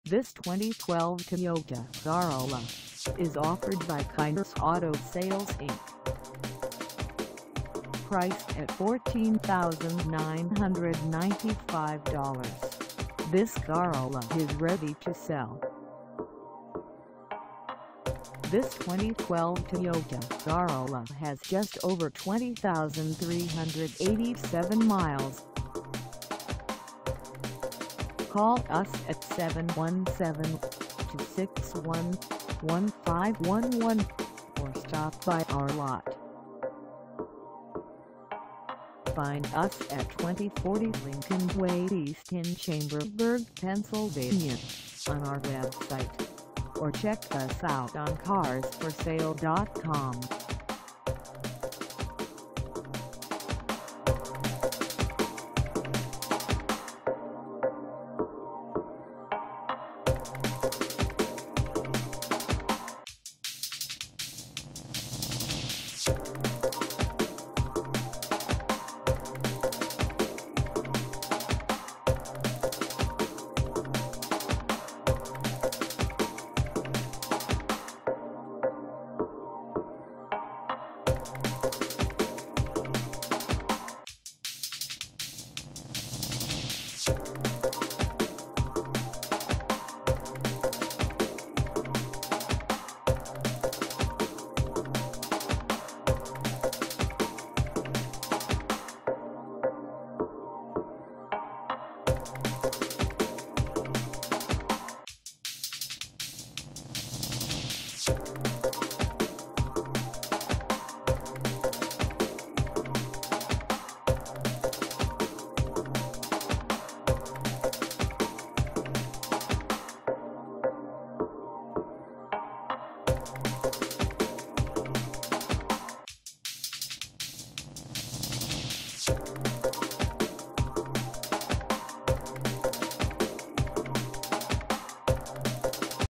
2012 Toyota Corolla LE 4dr Sedan 4A for sale in Chambersburg
This 2012 Toyota Corolla LE 4dr Sedan 4A is for sale in Chambersburg, PA 17202 at Kyners Auto Sales INC.
Engine: I4 1.8L I4
Transmission: Automatic 4-Speed
Color: Red
Mileage: 20387
Contact Kyners Auto Sales INC 2040 Lincoln Way E Chambersburg, PA 17202 to test drive this 2012 Toyota Corolla LE 4dr Sedan 4A today.